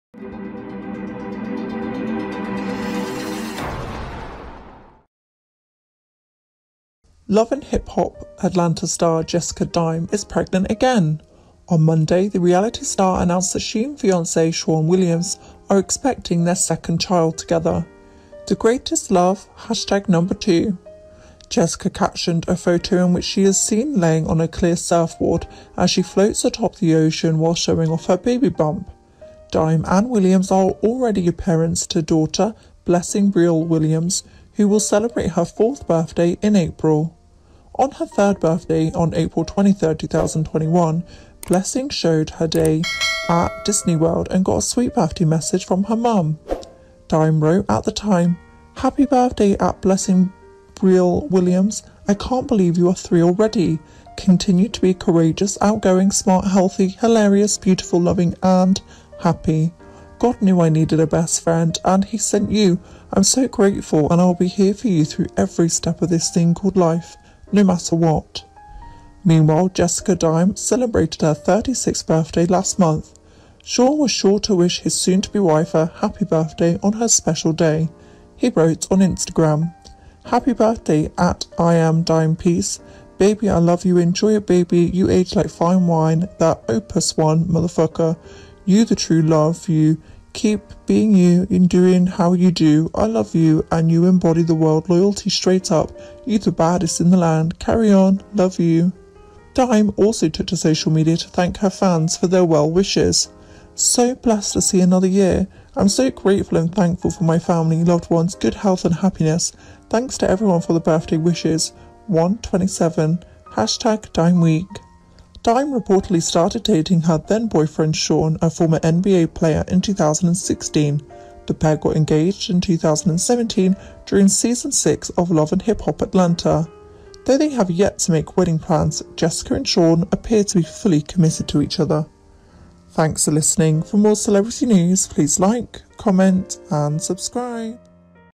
Love & Hip Hop Jessica Dime Expecting 2nd Child With Shawne Williams & Show Her Massive Baby Bump!
Jessica Dime is a main cast member on season seven of Love & Hip Hop: Atlanta. She appears as a supporting cast member from season four through six. Jessica Dime is pregnant again! On Monday, the reality star announced that she and fiancé Shawne Williams are expecting their second child together.

“The greatest love … #2,” Jessica captioned a photo in which she is seen laying on a clear surfboard as she floats atop the ocean while showing off her baby bump.